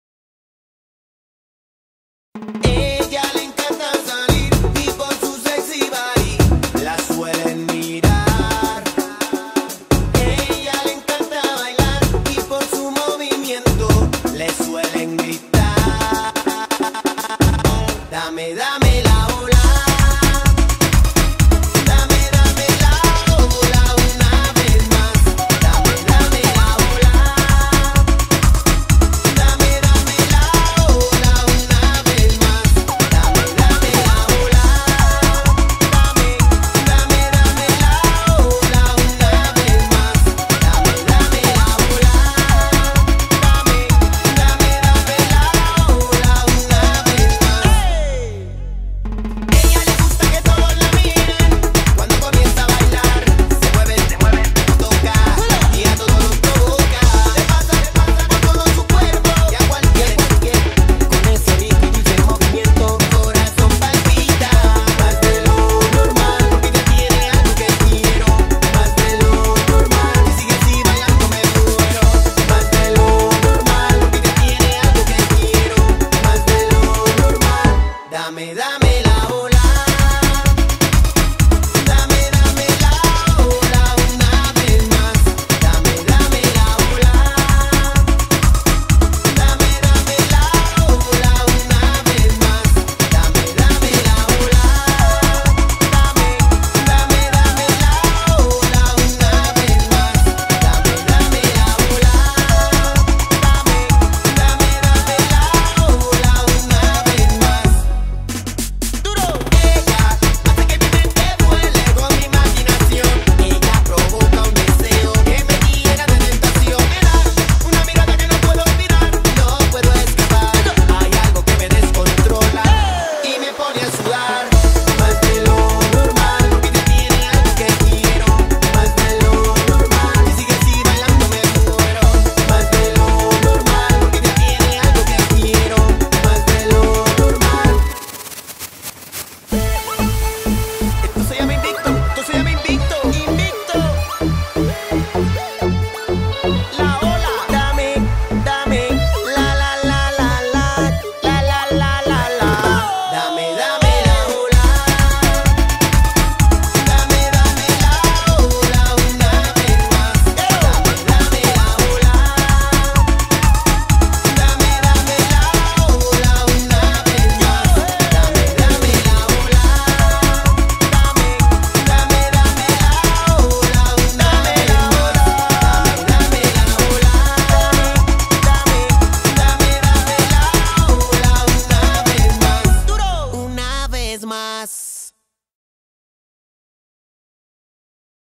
Zumba® Fitness with Juliana Lima
Heritage Days Celebration
Teen Night
Faribault, MN